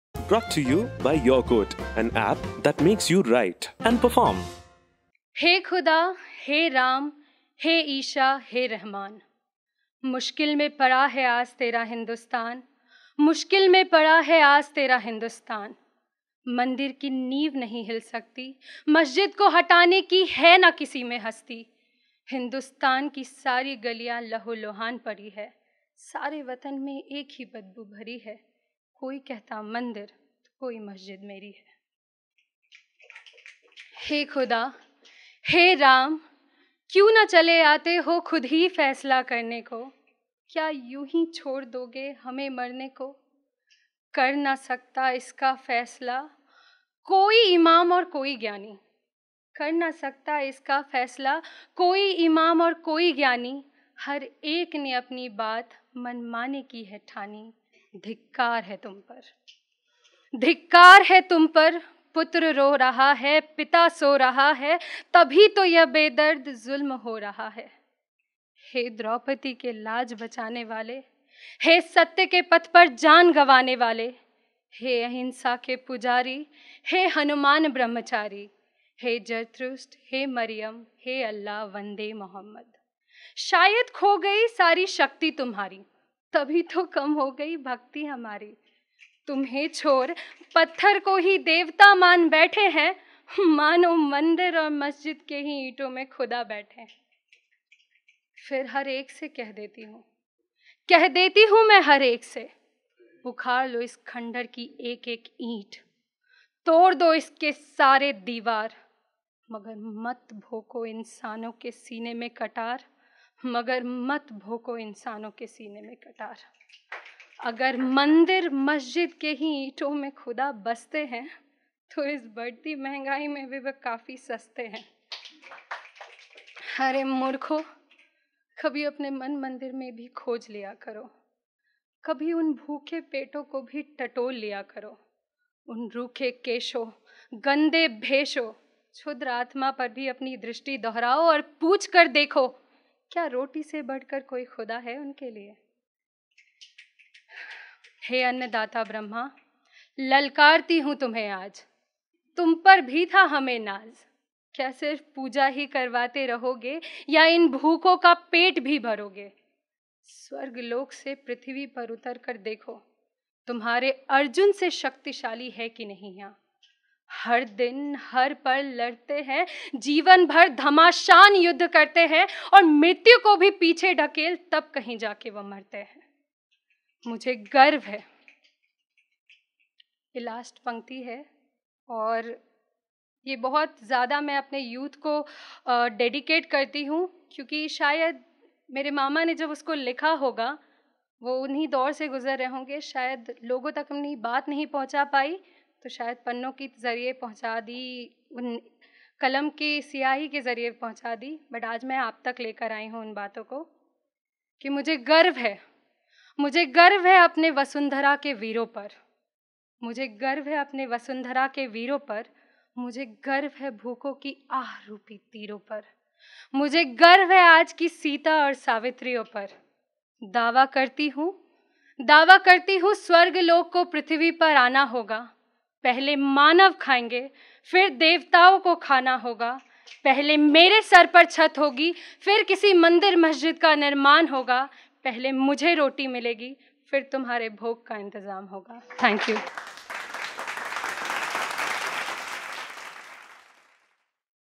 'Mandir Masjid' by Divya Lahiri | Hindi Poetry | YQ - Story Saturdays (Open Mic 5)
Venue: YourQuote Studio, Bank of India, Jeevan Bhima Nagar Main Road, Bengaluru